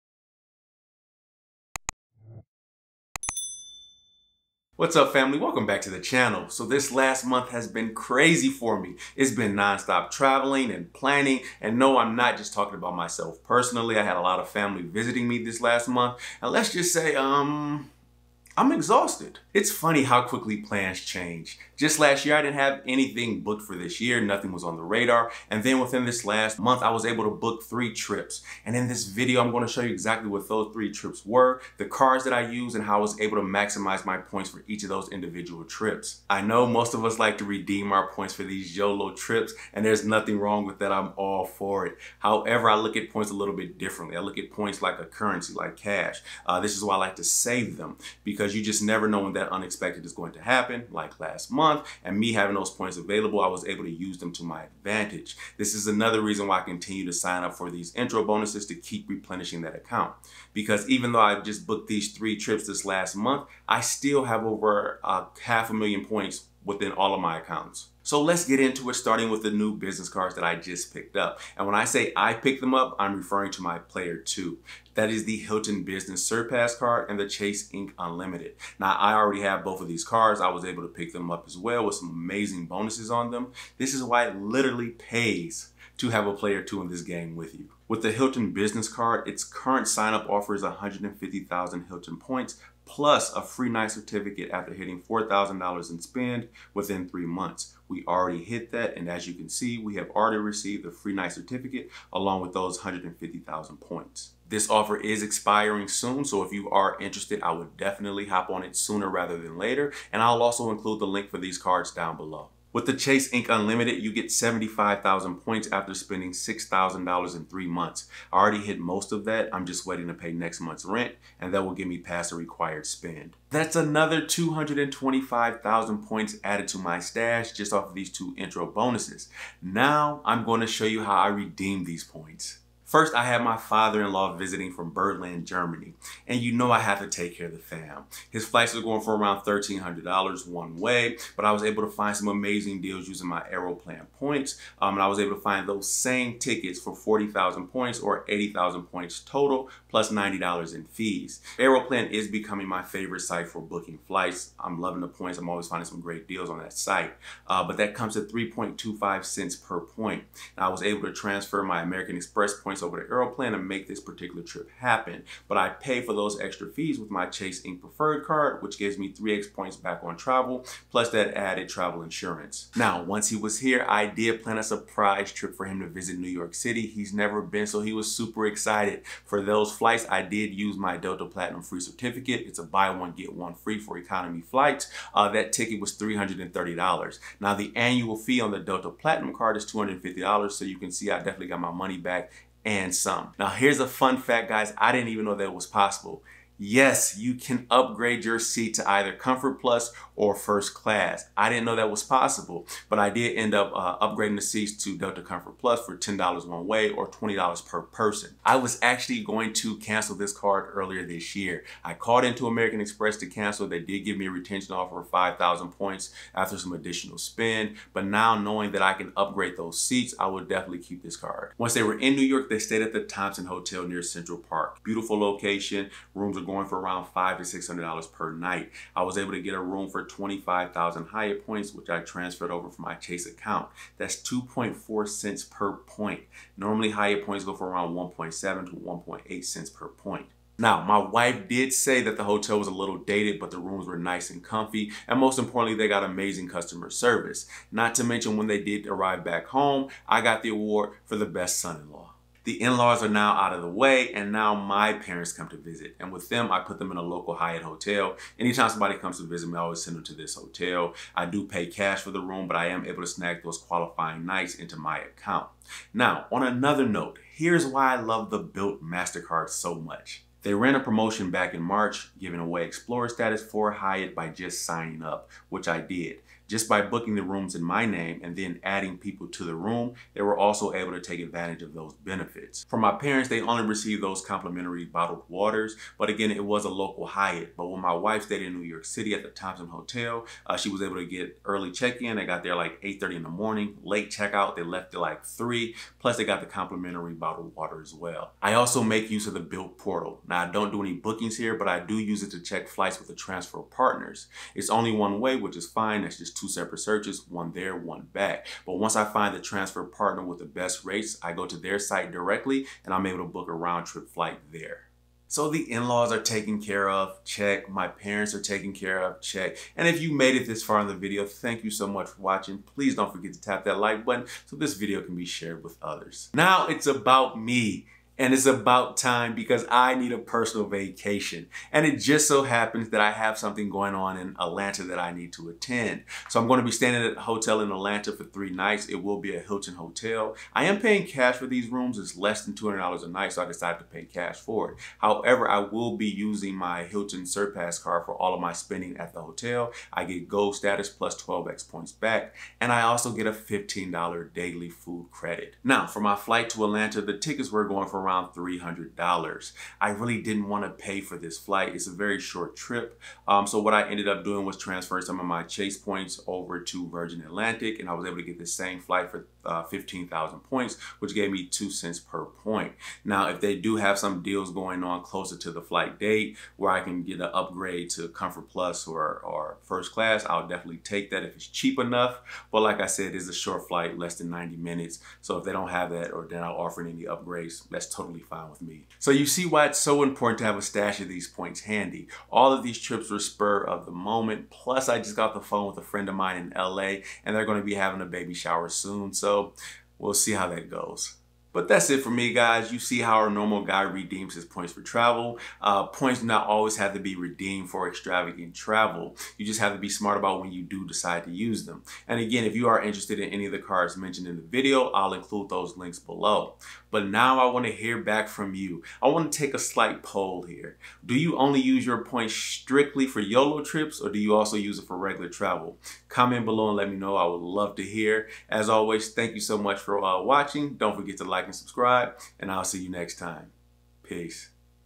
How To Redeem Credit Card Points For MAX Value (Beginner's Guide)
Find out how to redeem credit card points and rewards for maximum value. Credit card points are a form of currency, so make sure that you're redeeming them for as much value as possible.

Credit Card Links:
1.) Chase Ink credit card
2.) Capital One Savor credit card

00:00 Start here
01:29 Sign up bonuses
02:02 Ink Business credit card
03:04 Delta Certificate
In this video I break down how to redeem credit card points using all of the major credit cards, american express, chase, capital one, bilt rewards, and citibank. Whether you're using ultimate reward points, cit points, amex membership reward points or venture miles, you want to make sure that you're able to redeem at a high value. I show you examples of redemptions and which ones are the best travel credit cards.